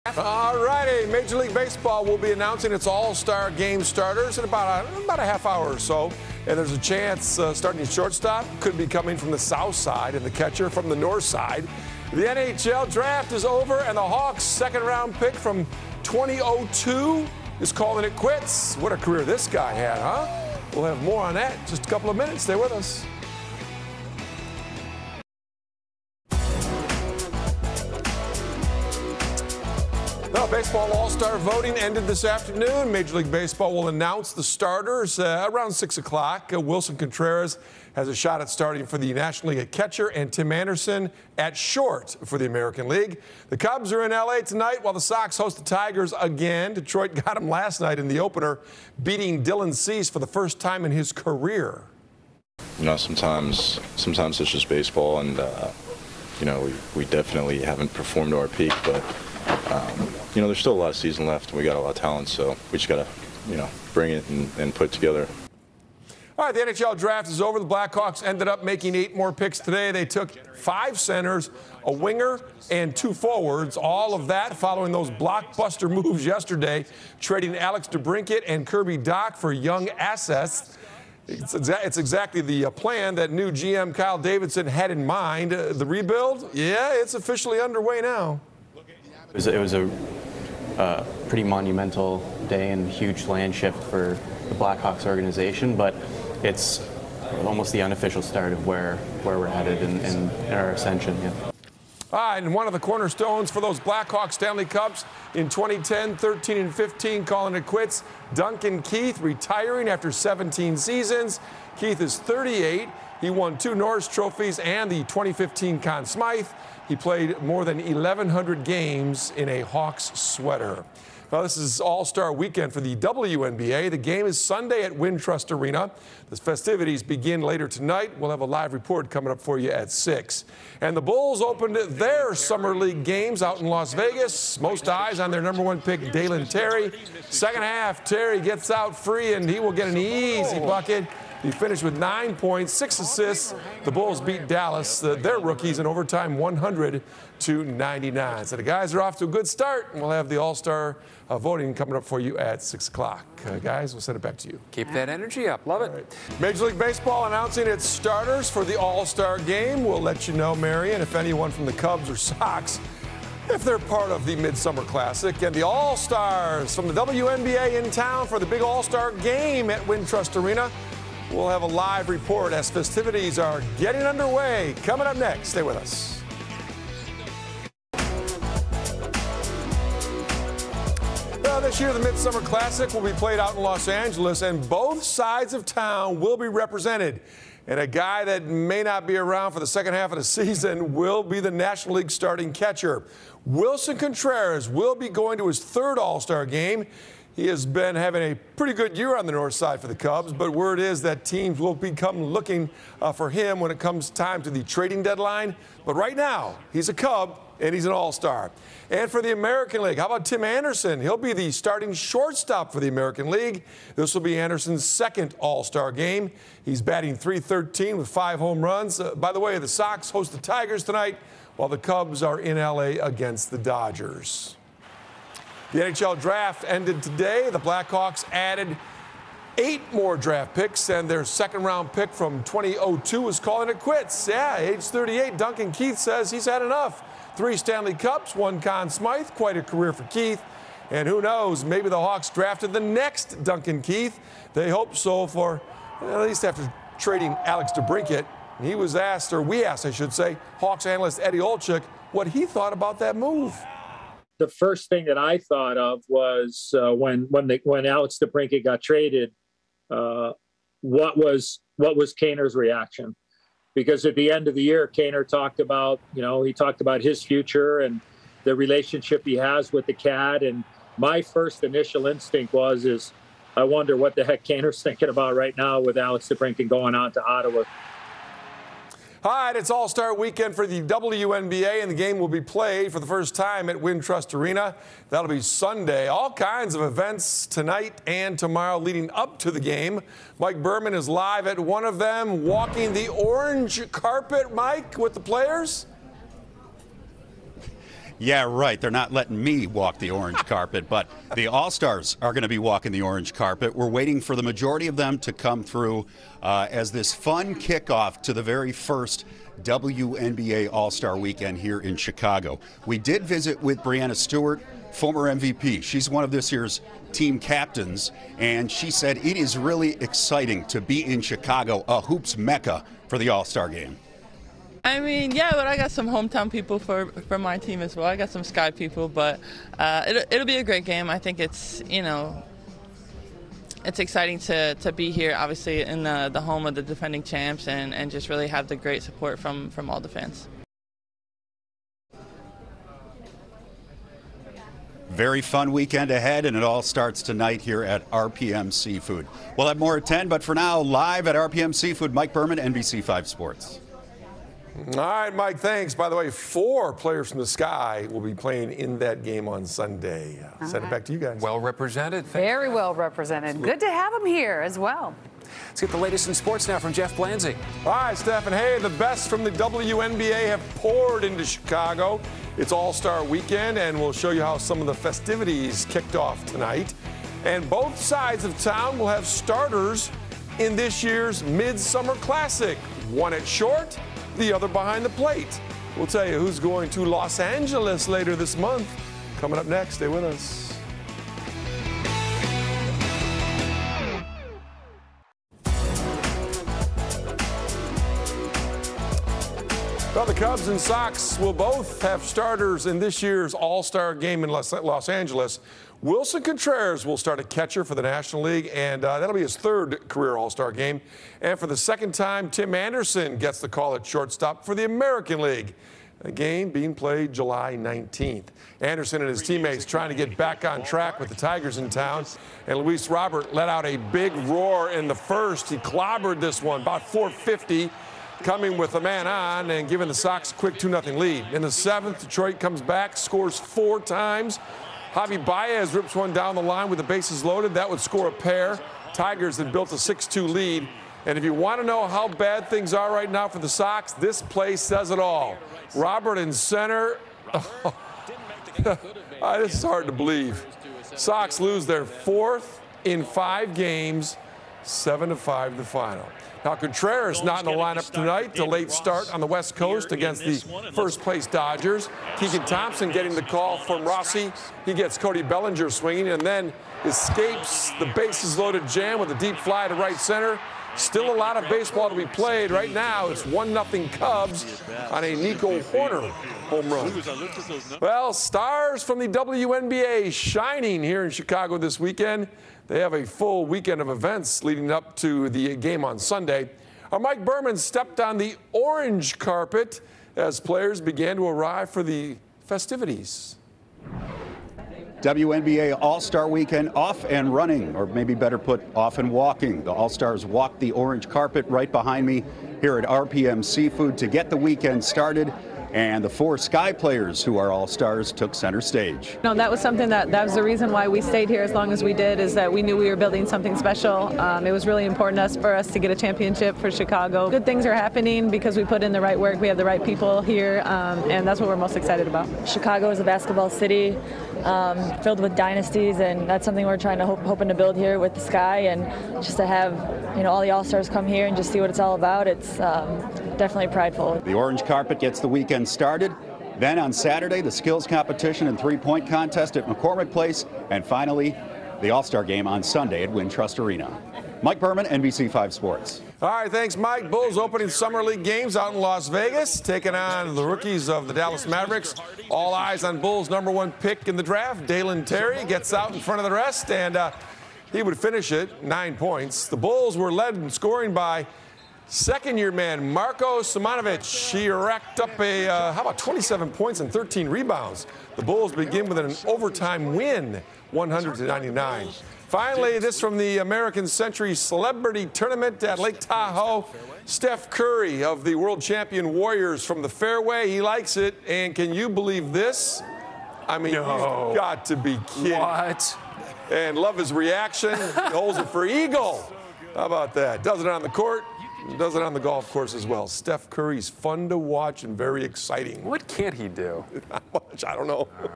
NBC5 - SPORTS TELECASTS - JUNE 8TH, 2022
Produced sports telecasts for 5pm, 6pm & 10pm news shows. Had remote elements with Mike Berman who was on-site at WNBA All-Star red carpet.